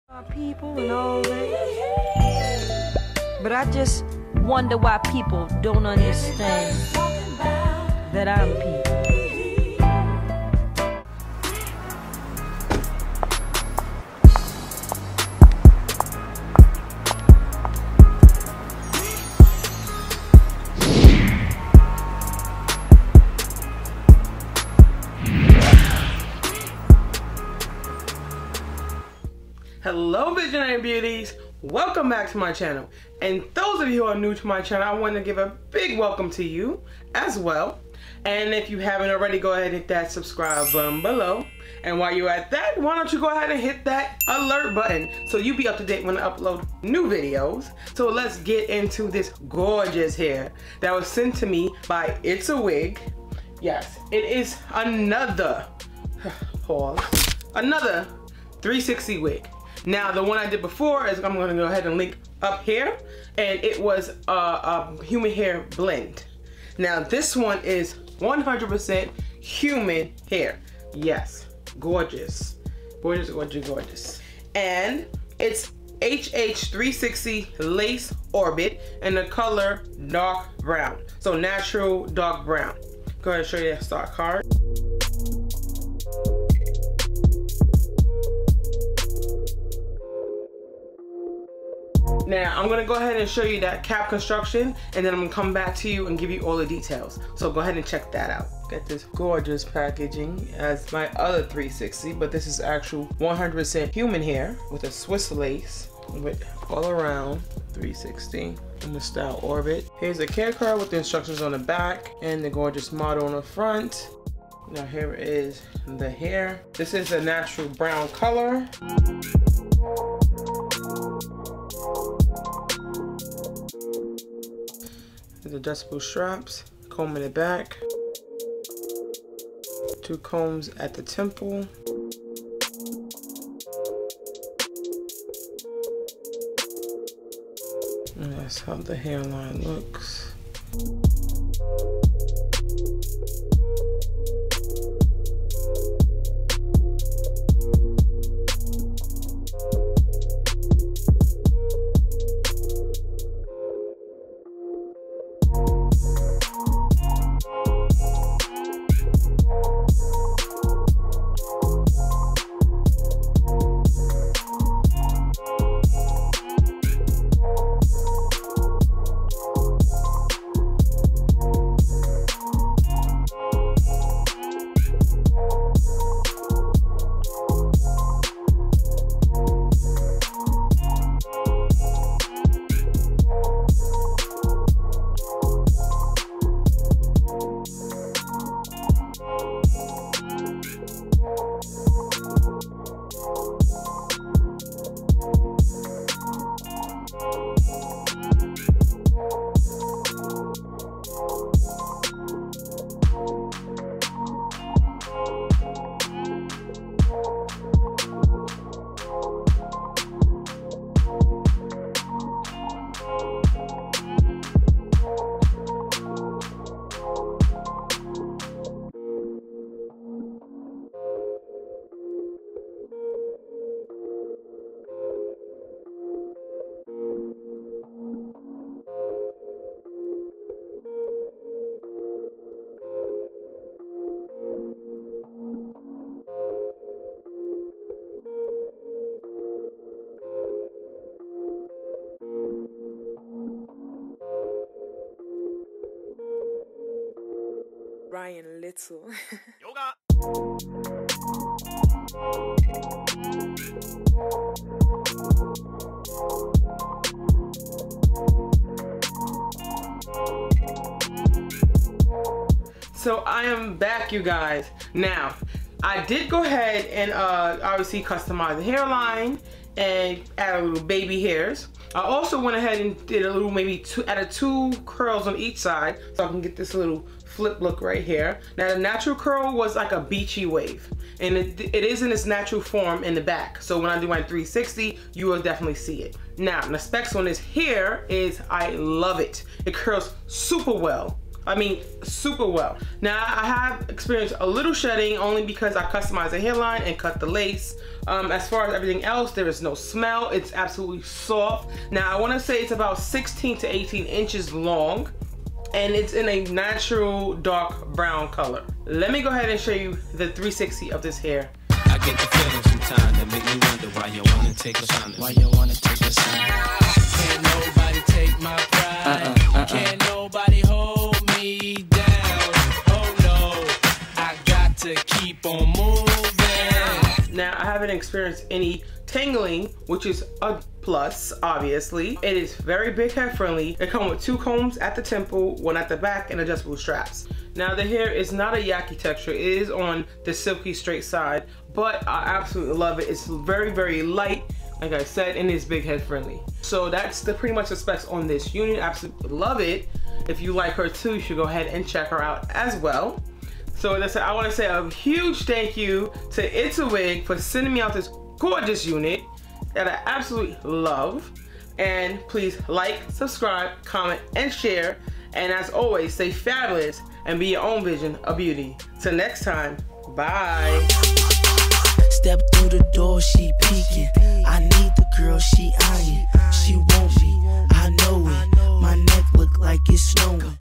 🔥😍It's A Wig | 100% Human Hair | HH 360 S Lace ORBIT D.Brown Wig review | Ft. it's a wig
👋🏽 VB's, This review is in collaboration with the itsawig101 campaign. I'm coming to you with my ✌🏽 360 All-Round wig by It's A Wig, but this time she's 100% Human Hair 😃😉 This wig is amazing!!! Video features packaging, cap construction, customizing hairline, cutting of lace, 360 view and my honest opinion.